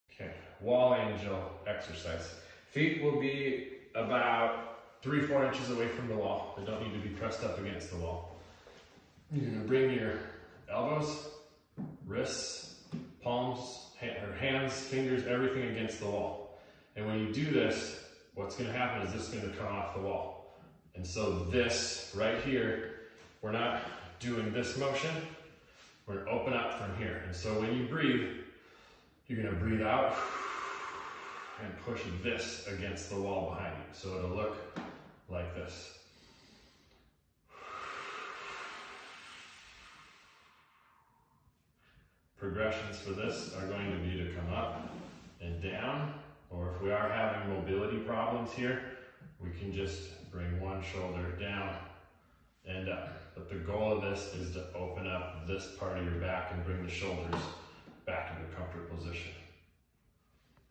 Wall Angel
Trying to keep wrists and fingers against the wall.  If unable to do this position, lay on the floor so gravity helps.  As you breathe out, "push" lower ribs into the wall. 4-5 repetitions, 3-5 times per day.  Great micro break during work day!